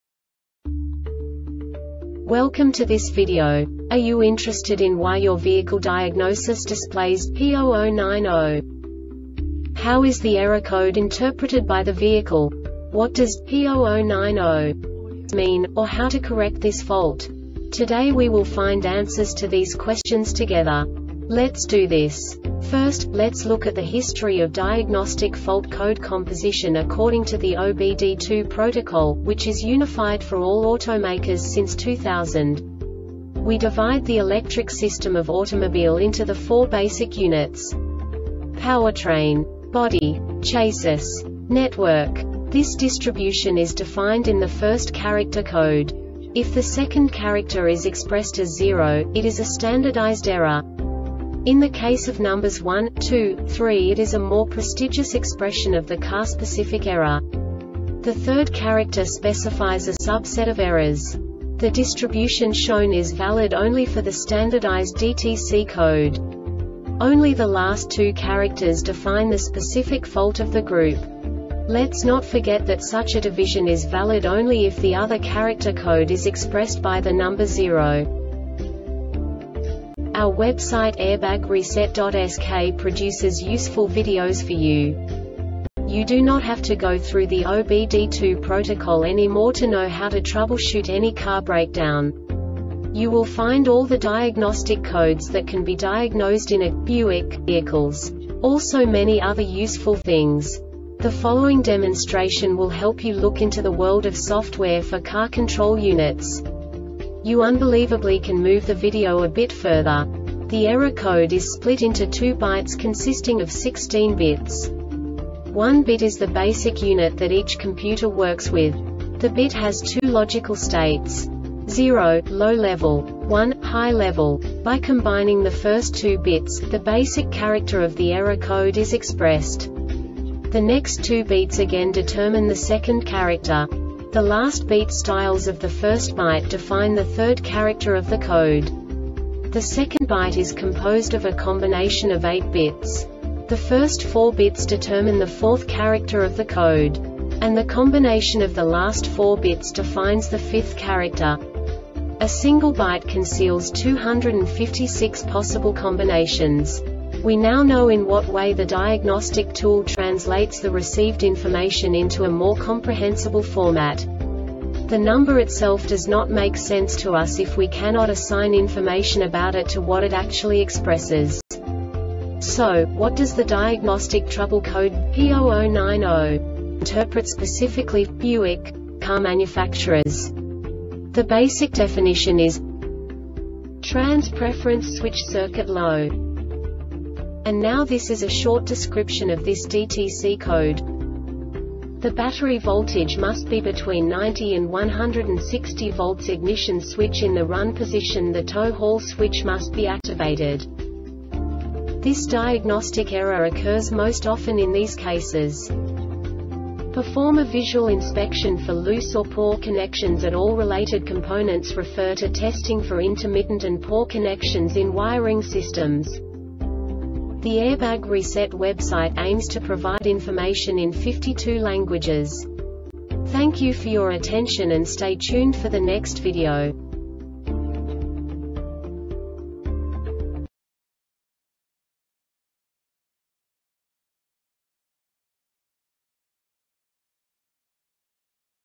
DTC Buick P0090 Short Explanation
Contents:
0:21 Basic DTC analysis according to OBD2 protocol standard.
1:48 Insight into programming
2:58 A diagnostically understandable description of the Diagnostic Trouble Code
3:05 Basic error definition

Make:
Buick
Code:
P0090
Definition:
Trans Preference Switch Circuit Low
Description:
The battery voltage must be between 9.0 and 16.0 volts.Ignition switch in the RUN position.The Tow or Haul switch must be activated.
Cause:
Perform a visual inspection for loose or poor connections at all related components.Refer to Testing for Intermittent and Poor Connections in Wiring Systems.